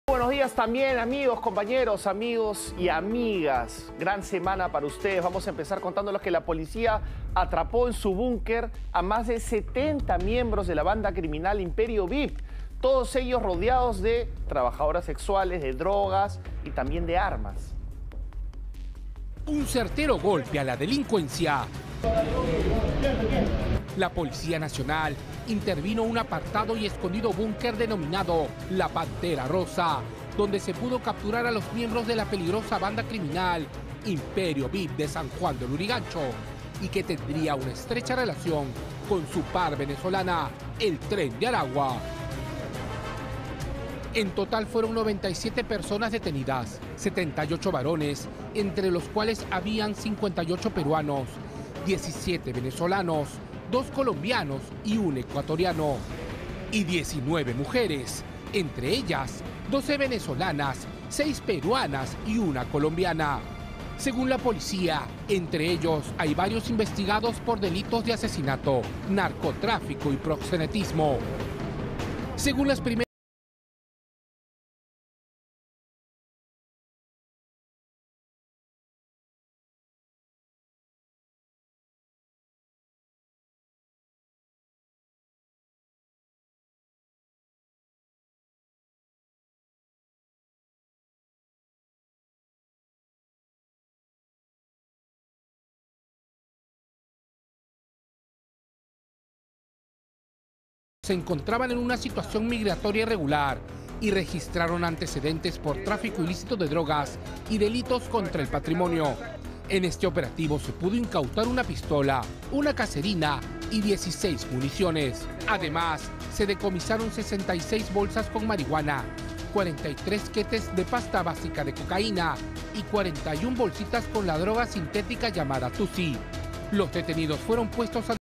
SJL: PNP interviene búnker conocido como 'Pantera Rosa' y detiene a casi 100 personas
Delincuentes pertenecen a la banda criminal Imperio VIP, relacionado con el "Tren de Aragua".